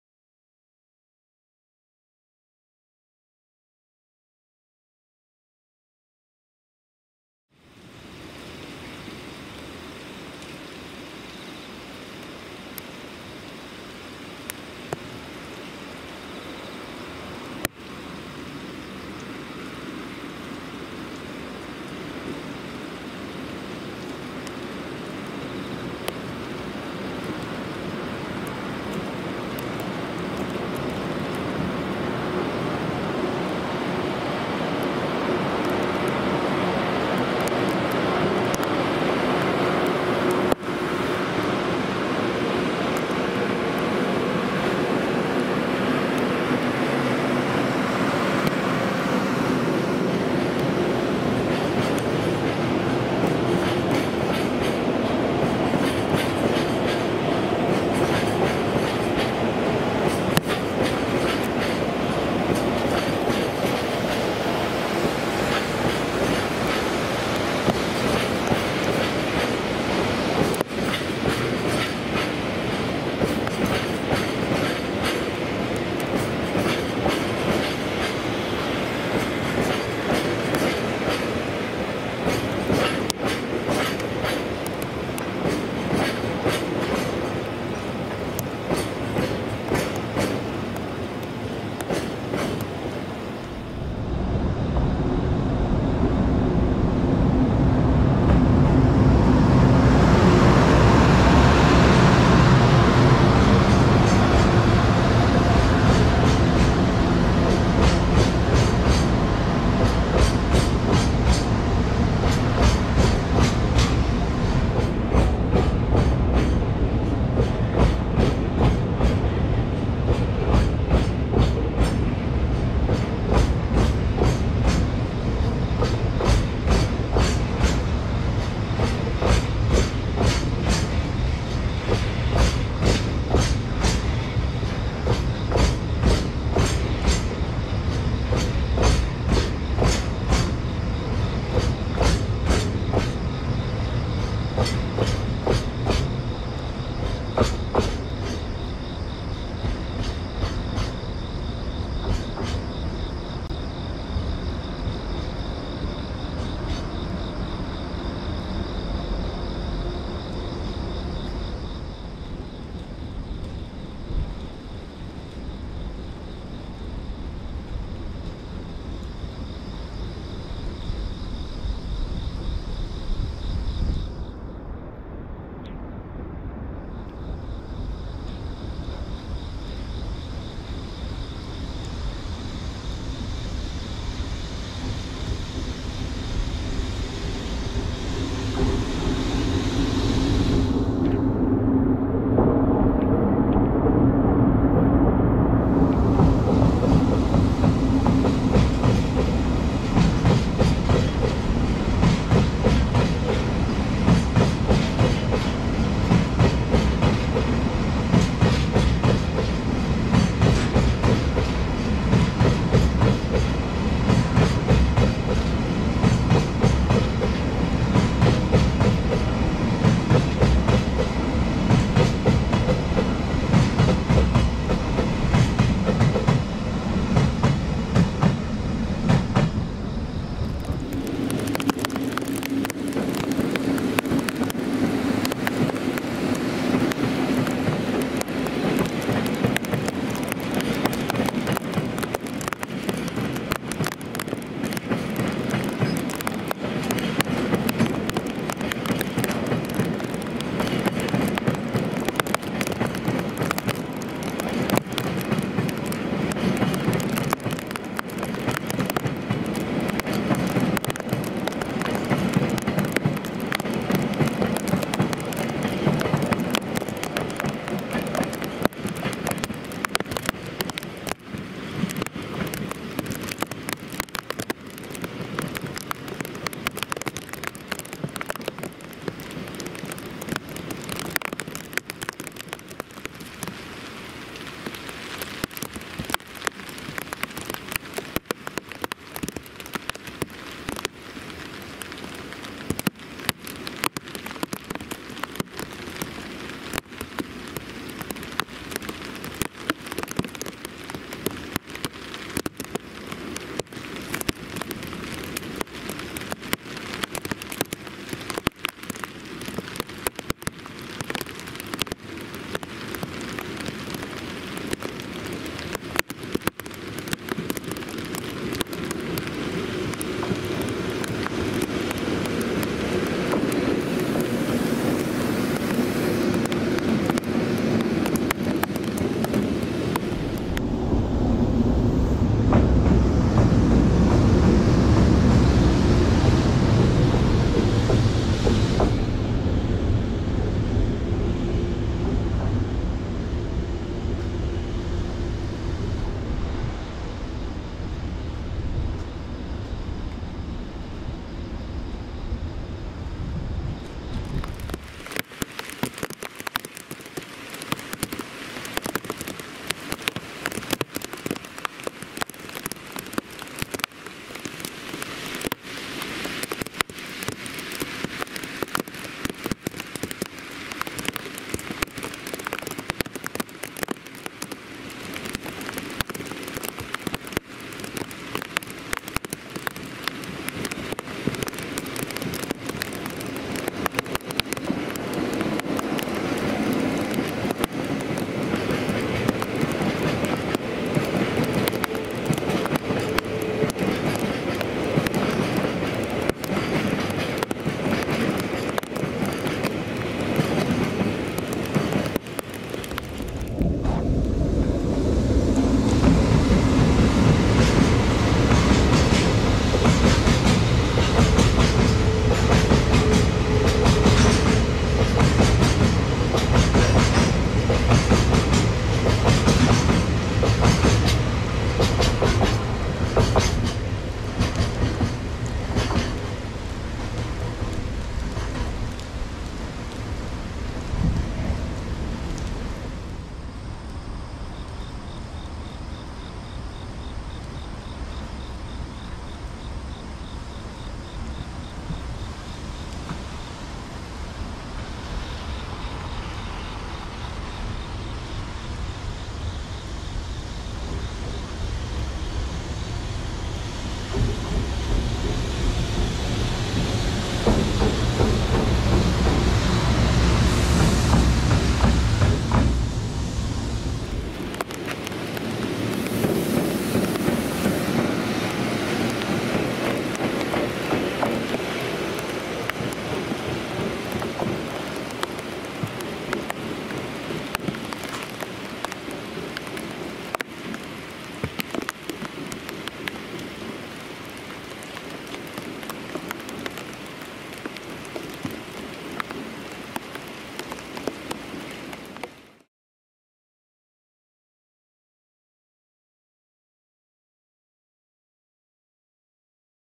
【貨物】2021/09/18 高島線 石油輸送 5-6時(Takashima Line. Oil transportation. 5-6 o'clock. 4K)
東海道貨物線界隈の貨物列車中心に動画紹介しています。

The video is introduced mainly for freight trains in the Tokaido freight line areas.

撮影日：2021/09/18
路線名：高島線
列車番号, 列車情報：
- 81レ EH200-3 + タキ15B
- 5560レ EF65 2074 + タキ14B + コキ2B
- 83レ EH200-8 単機
- 8097  EF65 2074 + タキ10B
- 8470  EF210-328 単機

撮影情報：高島線。石油輸送。5-6時。4K撮影。

Recording date : Sep 18 2021.
Route name : Takashima Line.
Operation number & Train information
- 81, EH200-3 + TAKI 15B
- 5560, EF65 2074 + TAKI 14B + KOKI 2B
- 83, EH200-8 single
- 8097, EF65 2074 + TAKI 10B
- 8470, EF210-328 single

(TAKI is a  tanker freight car.)
(KOKI is a container freight car.)

Recorded information : Takashima Line. Oil transport. 5-6 o'clock. Taken at 4K.